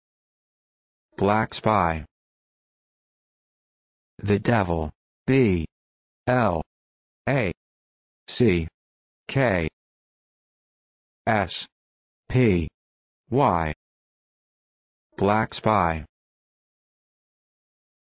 BLACK SPY Meaning - The Secret Language of Rogues
BLACK SPY is a word in The Secret Language of Rogues. Its Meaning is as Below:

The devil.

To Learn More about The Secret Language of Rogues, Look at the Book "Vocabulum; or, The Rogue's Lexicon", Written by George W. Matsell.

You can Find Related Videos on the WikiBookie Channel.